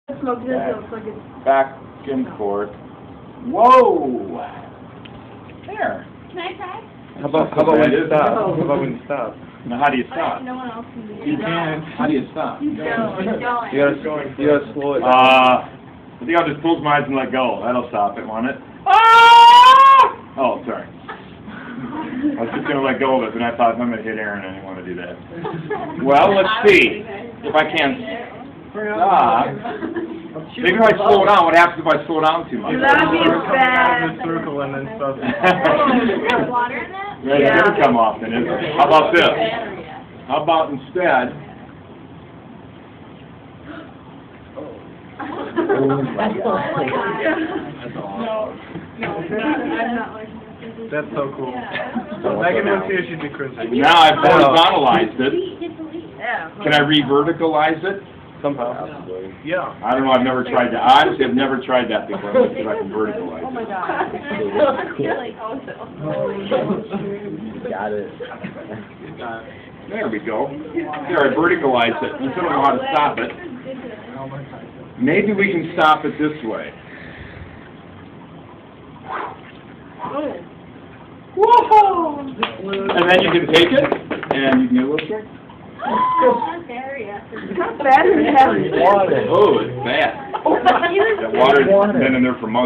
Centripetal Force
Swinging a cup of water overhead with a platform